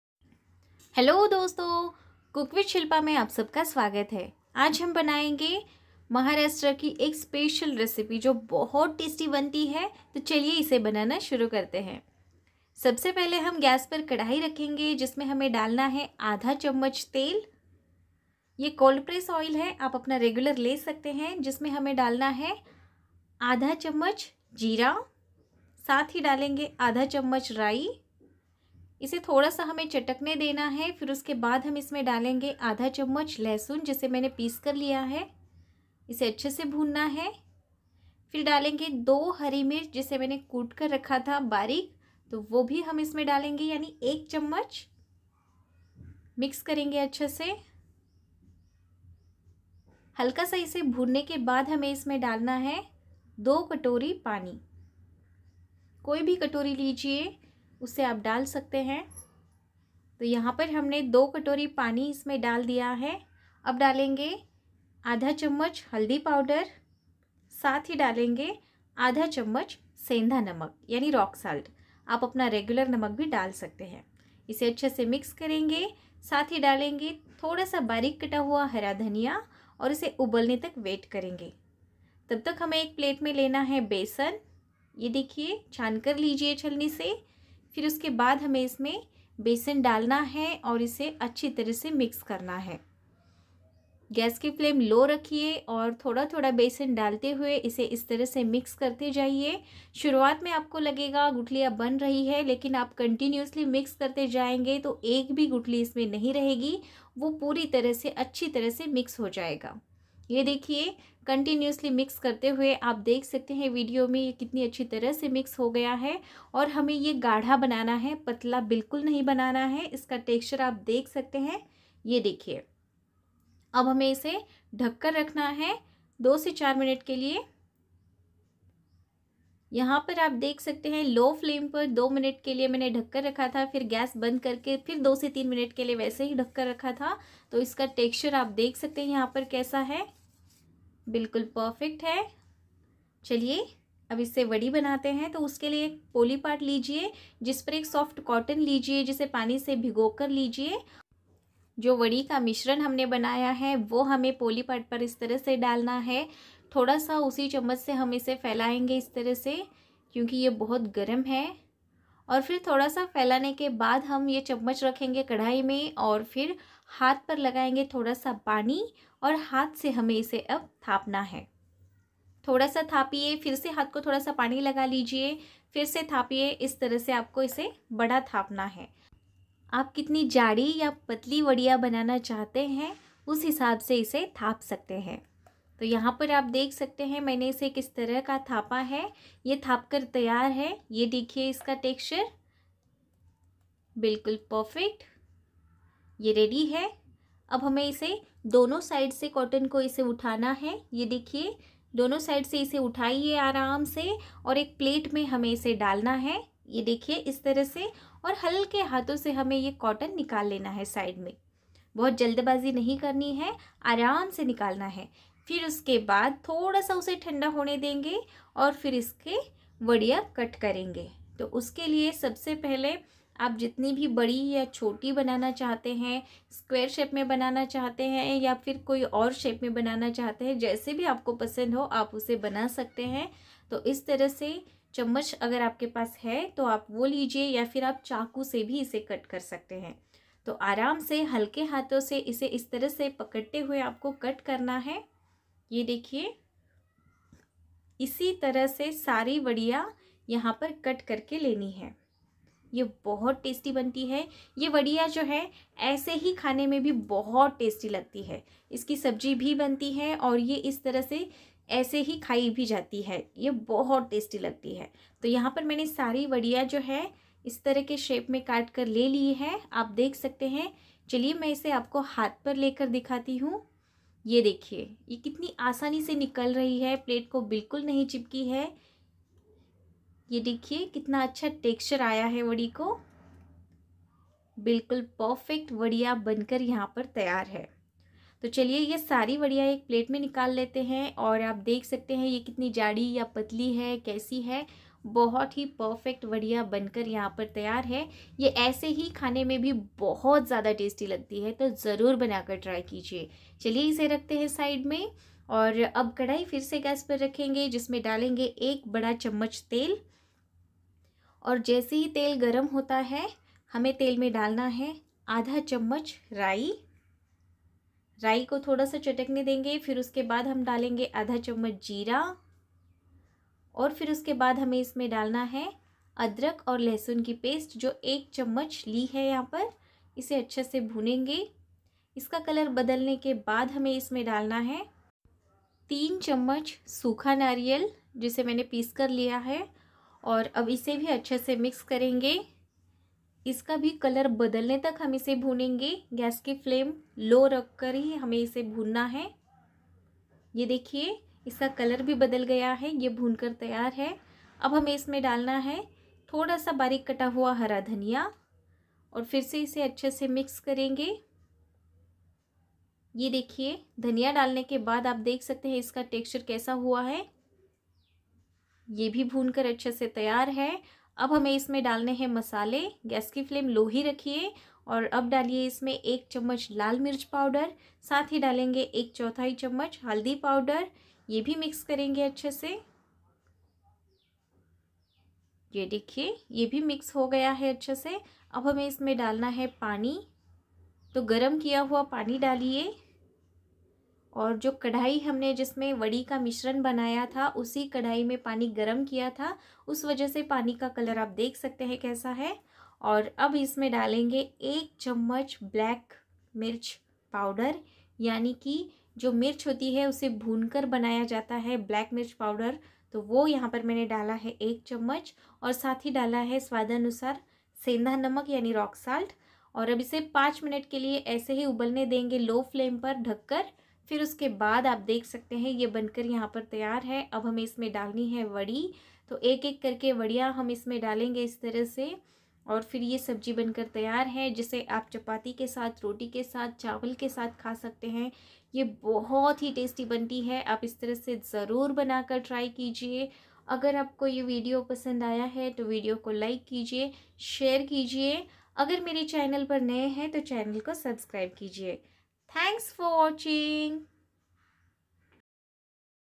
महाराष्ट्रीयन स्टाइल वडी रेसिपी
Maharashtrian style vadi ki sabji recipe

Ingredients.
1/2 = चम्मच तेल
1/2 = चम्मच राई
1/2 = चम्मच जिरा
   1 = चम्मच हरीमिर्च पेस्ट
1/2 = चम्मच हल्दी
   2 = कटोरी पाणी
      = बेसण
      = सेंधा नमक
      = हरा धनिया

   1 = चम्मच तेल
1/2 = चम्मच राई
1/2 = चम्मच जिरा